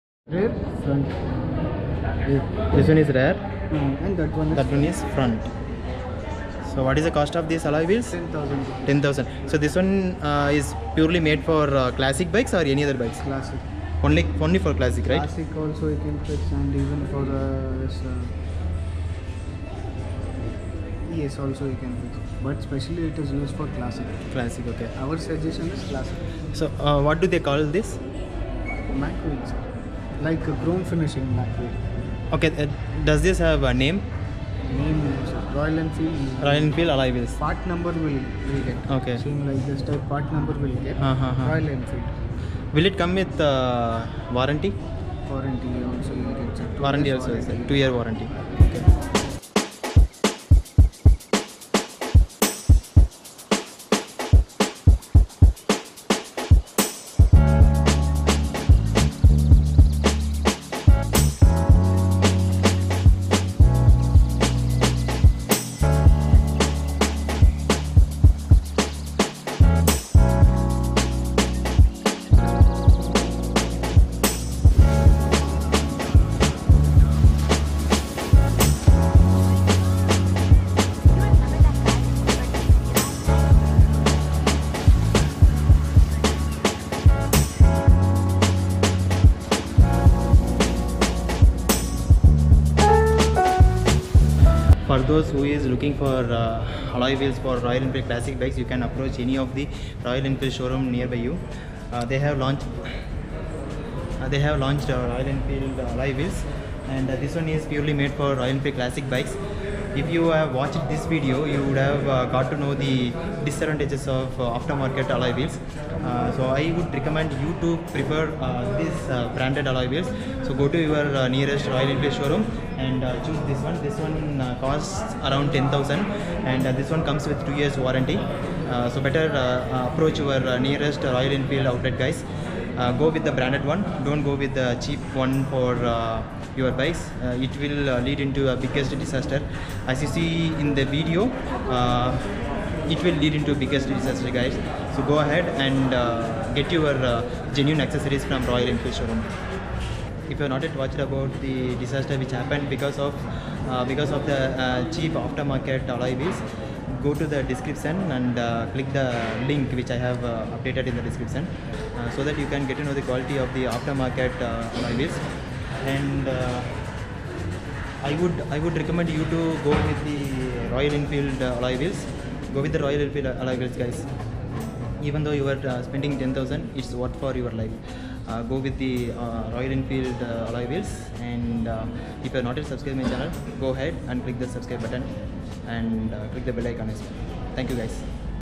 TUBELESS ALLOY WHEELS FROM ROYAL ENFIELD FOR ALL CLASSIC BIKES WITH 2 YEARS WARRANTY
ROYAL ENFIELD CLASSIC BIKE FELL ON THE ROAD || DON'T EVER FIX THIS ALLOY WHEELS FOR YOUR BIKE

Cameras I use to record:

Nikon D5200 with Kit lens & Prime lens
SJCam SJ7 star
GoPro Hero 7 Black
OnePlus 5 mobile camera